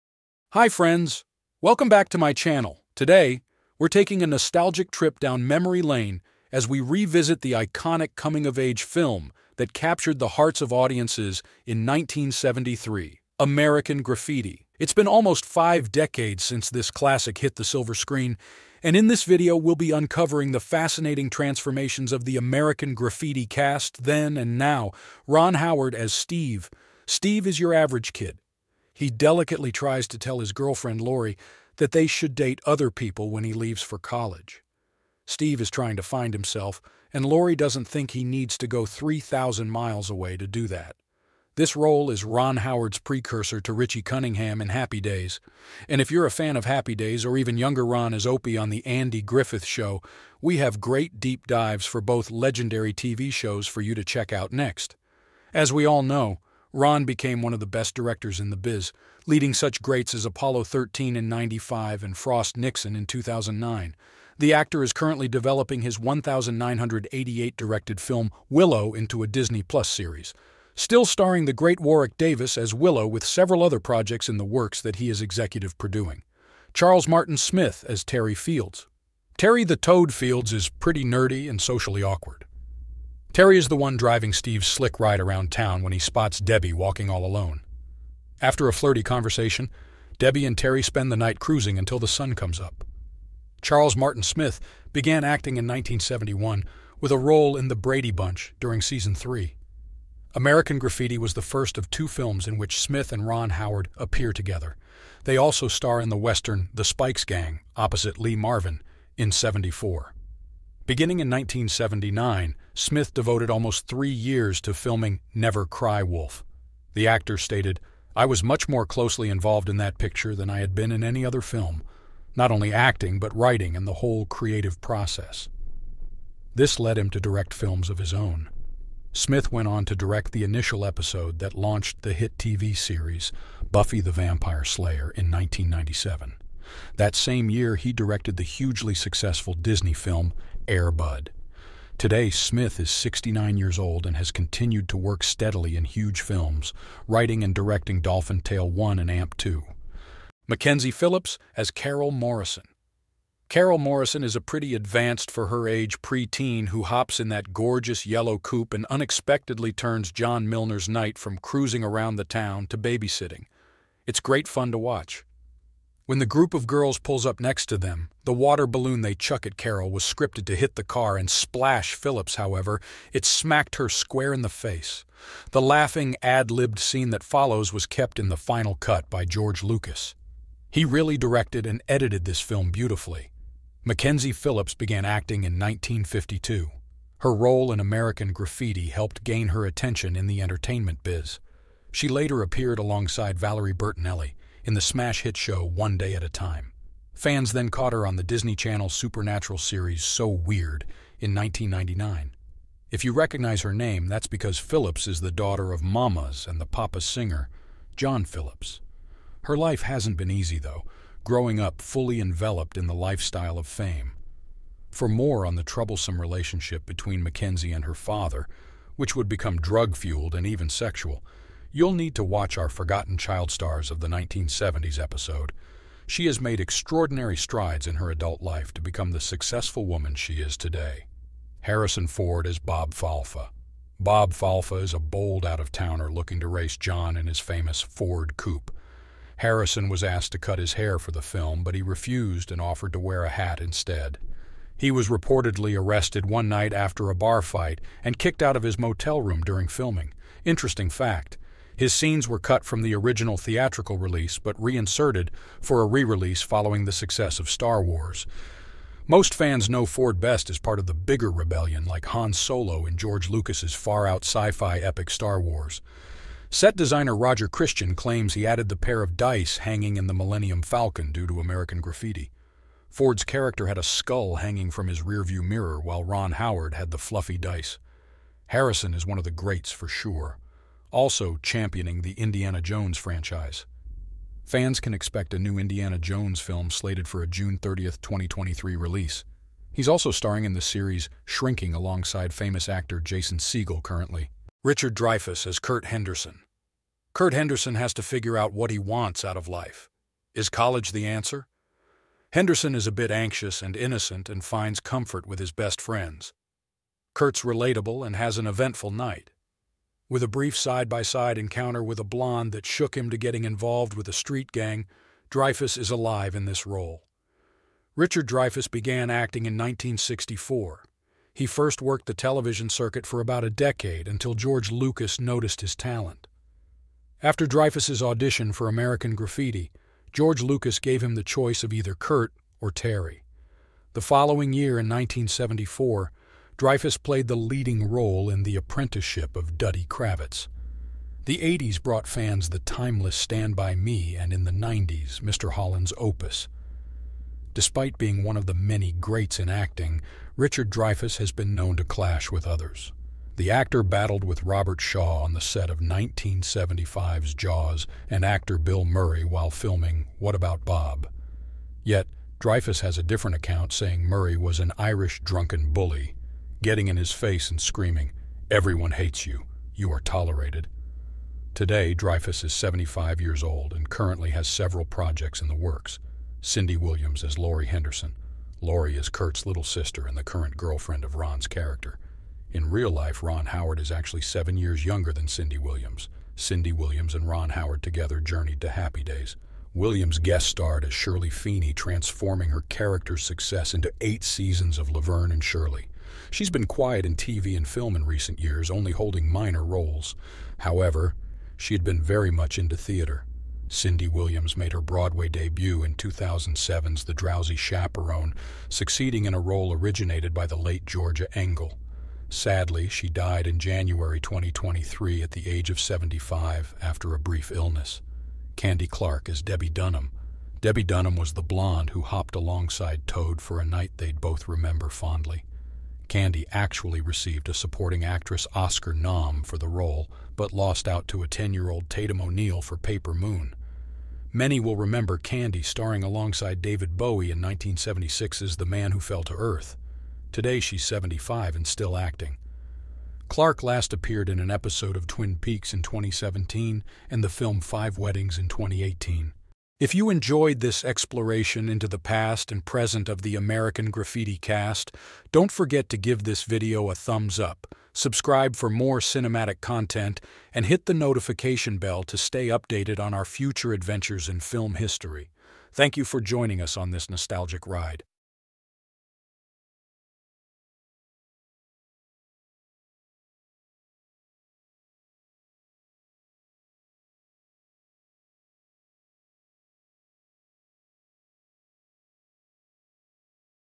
AMERICAN GRAFFITI 1973 Cast The actors have aged horribly!!
In this video you`ll see how the actors of the movie AMERICAN GRAFFITI have changed. You`ll know their real names and age. I will show you how they looked then, and what`s with them now, in 2023.
AMERICAN GRAFFITI Real Name and Age;
AMERICAN GRAFFITI Then and Now;
AMERICAN GRAFFITI How They Changed;

👉American Graffiti is a 1973 American coming-of-age comedy-drama film directed by George Lucas, produced by Francis Ford Coppola, written by Willard Huyck, Gloria Katz and Lucas, and starring Richard Dreyfuss, Ron Howard, Paul Le Mat, Harrison Ford, Charles Martin Smith, Cindy Williams, Candy Clark, Mackenzie Phillips, Bo Hopkins, and Wolfman Jack. Set in Modesto, California, in 1962, the film is a study of the cruising and early rock 'n' roll cultures popular among Lucas's age group at that time.